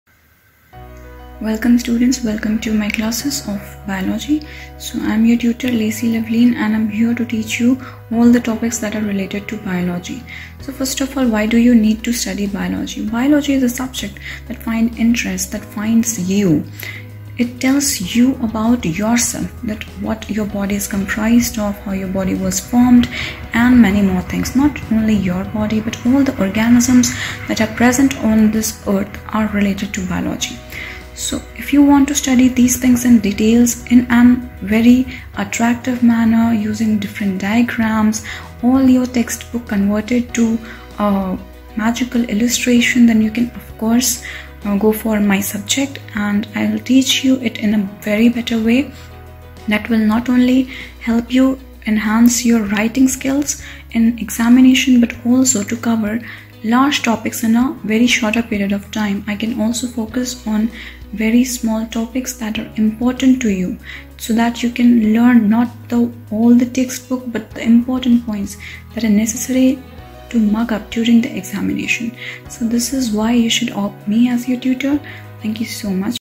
Know Your GuruQ Tutor : Lacy
"Meet Ms Lacy, a GuruQ certified Biology tutor. Lacy is just beginning her career in teaching but her dedication towards her students is unmatchable. Her approach of using visuals during the class makes her students feel engaged and her class interesting.

To book your classes with Lacy, downlaod the GuruQ app now!